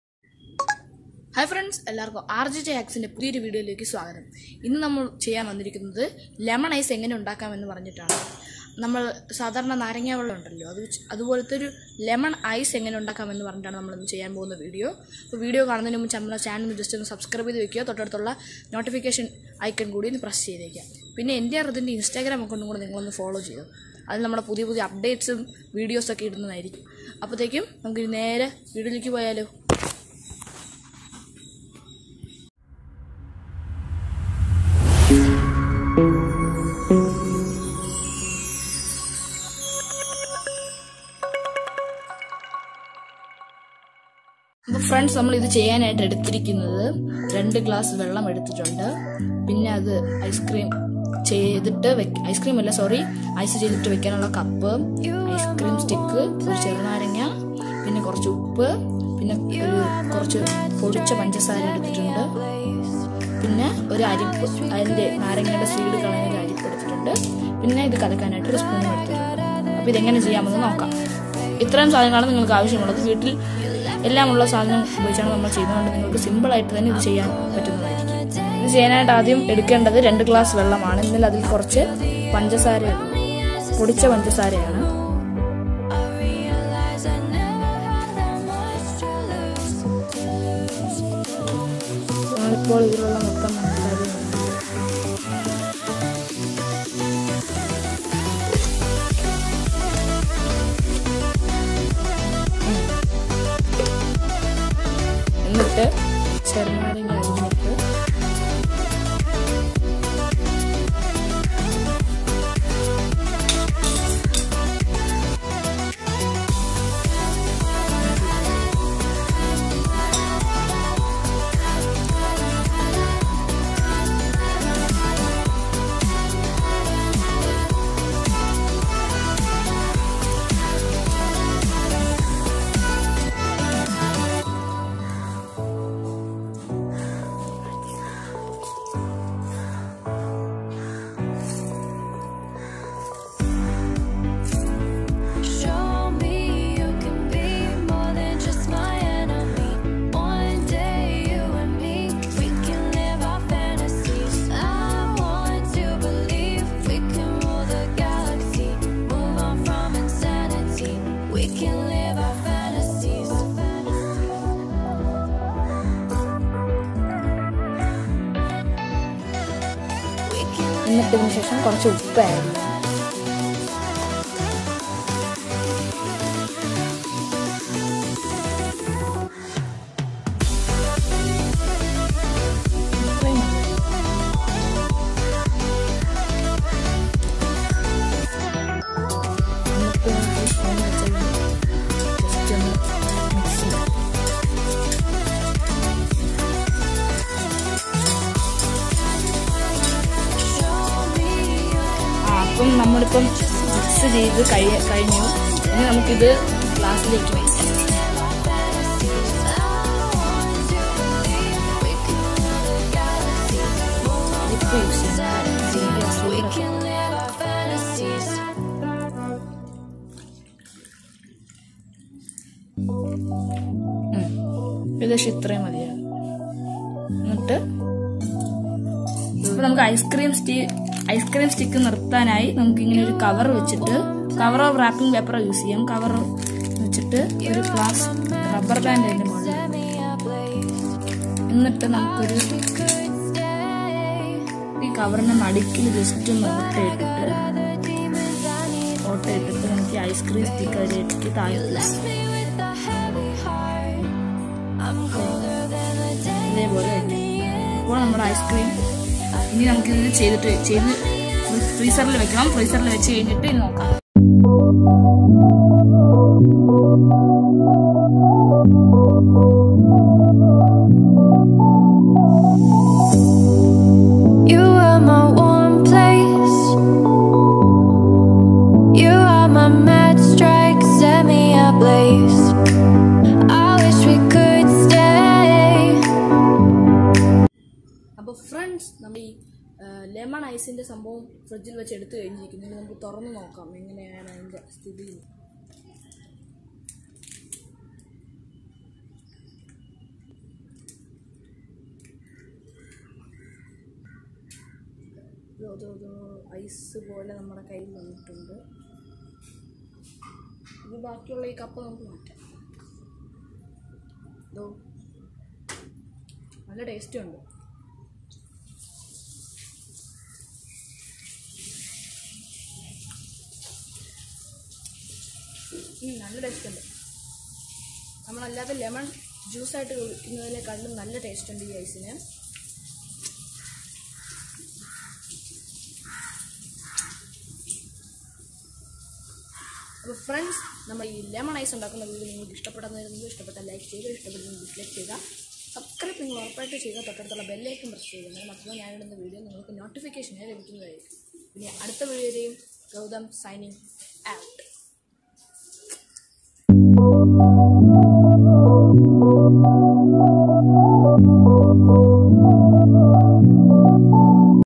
നാരങ്ങ ഐസ് | HOW TO MAKE LEMON ICE AT HOME🍋🍋 | IN MALAYALAM | RJJ HACKS
In this Video I am Making Lemon Ice
SO HOPE YOU ENJOYED THE VIDEO
In this Video I am making Invisible Ink

Follow Rithin on instagram :-

Follow RJJ hacks on instagram :-

rjj_hacks_yt

Appo Adutha video varekkum bye.